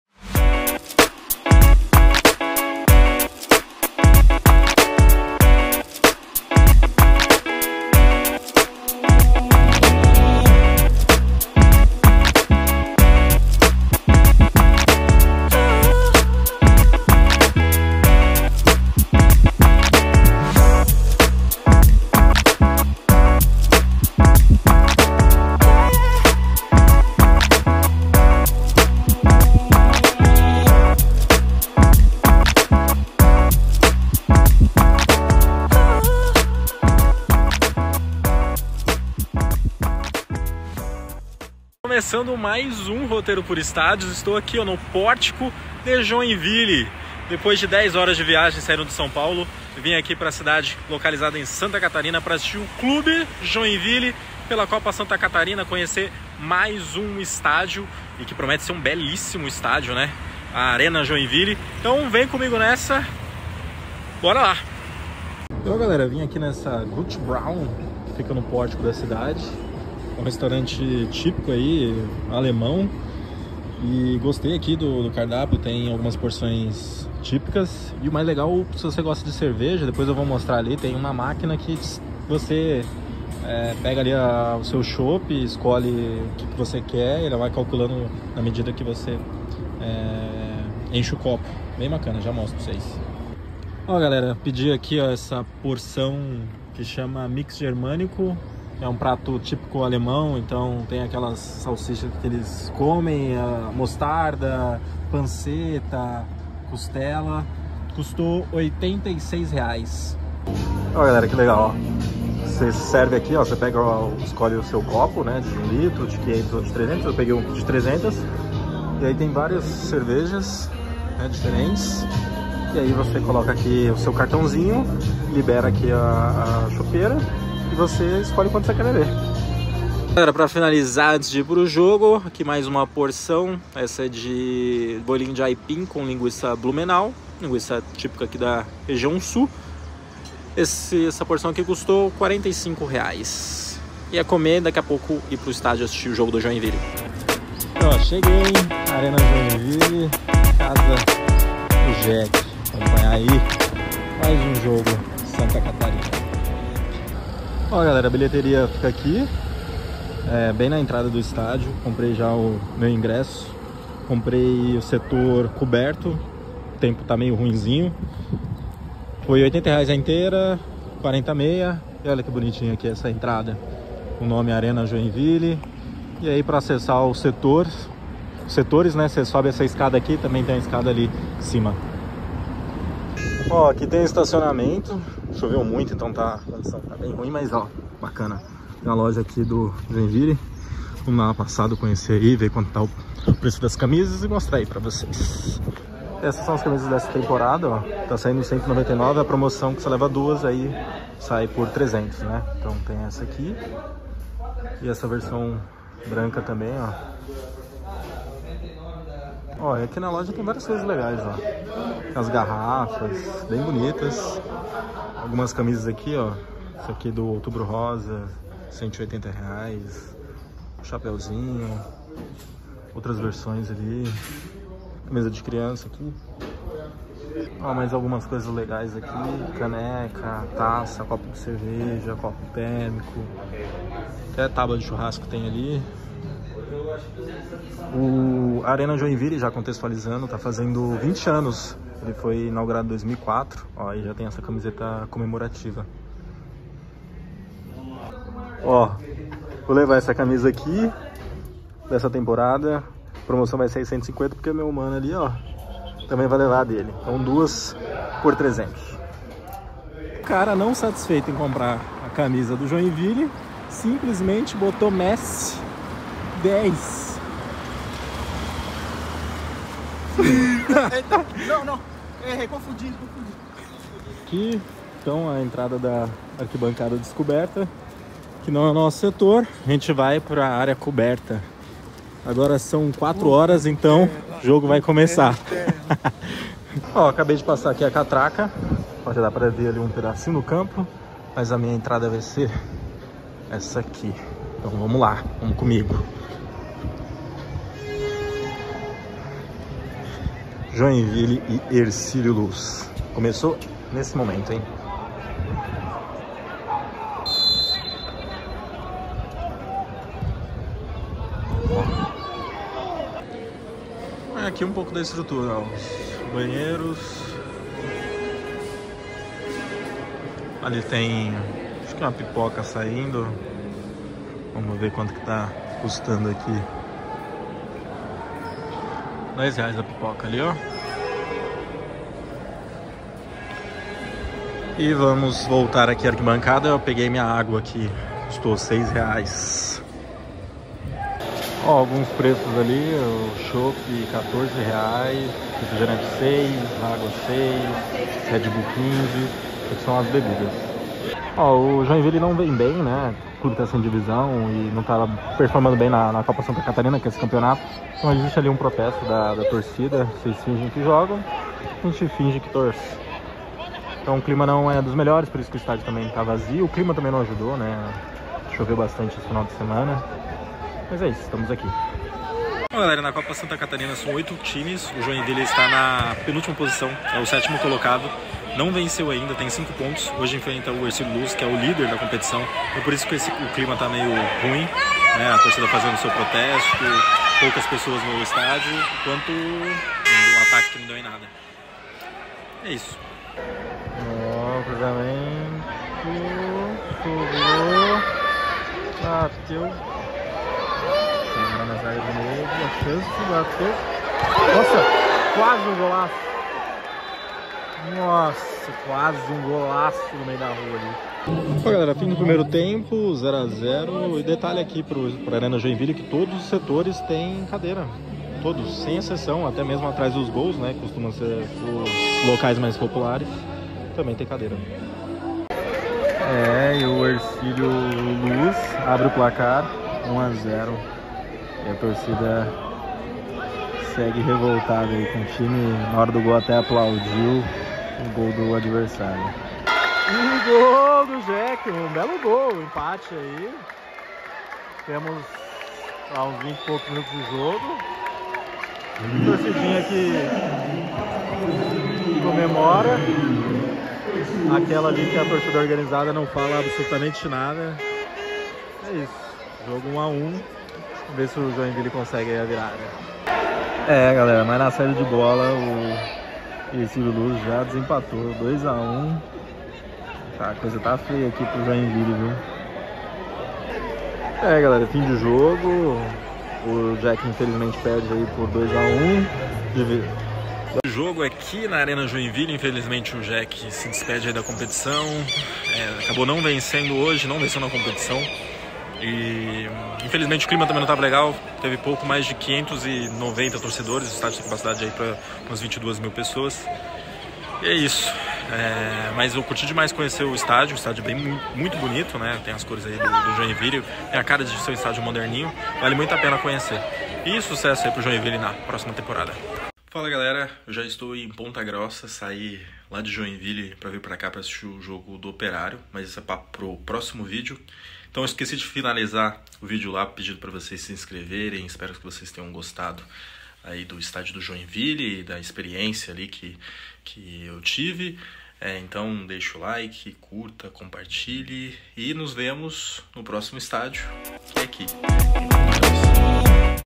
CONHECENDO A ARENA JOINVILLE E ASSISTINDO UM JOGO DO JEC | EPISÓDIO 67 🏟️
“Vocês fingem que jogam e nós fingimos que torcemos.”

Cômico! Escolhi o jogo em que a torcida do JEC protestou para conhecer a linda Arena Joinville. O clube não está em boa fase nos últimos anos, especialmente nesta Copa Santa Catarina, onde ainda não venceu nenhum jogo 🫣

O estádio, por outro lado, é excelente! Localização ótima, todos os setores com cadeiras, arquibancada bem próxima ao campo, área reservada para torcedores com deficiência, e uma loja com itens oficiais do tricolor.

Em 2025, o Joinville estará de volta ao cenário nacional, e espero retornar para ver a Arena lotada ⚫️🔴⚪️

⚽️ Joinville 1 x 2 Hercílio Luz
🏆 Copa Santa Catarina
🏟️ Arena Joinville
📍 Joinville, SC
🎫 R$ 80,00 (inteira)